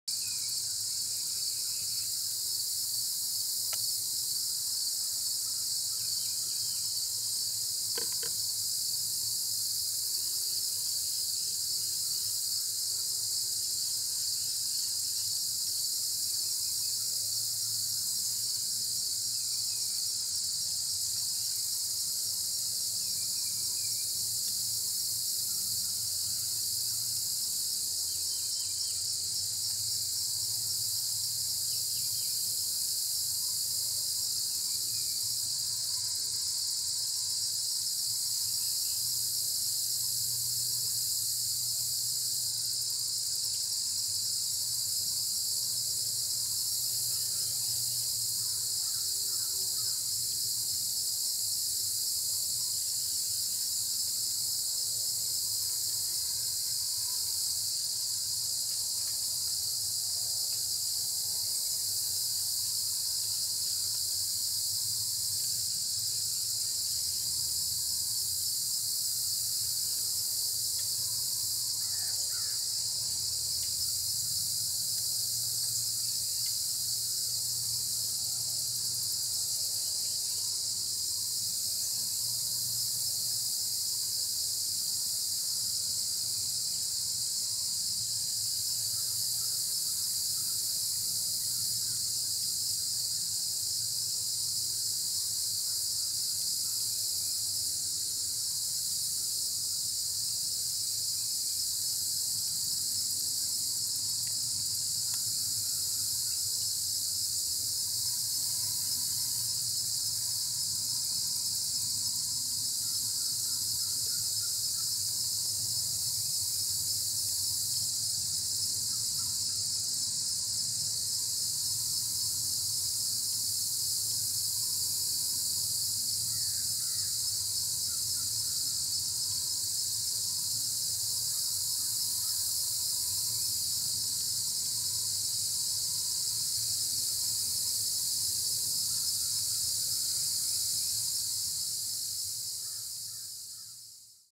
Banded Tussock Moth caterpillar on the move (Halysidota tessellaris)
All in real time. On an Elm tree. Host plants: Alder, Ash, Birch, Blueberry, Chestnut, Elm, Grape, Hackberry, Hazel, Hickory, Oak, Walnut, Willow and many others.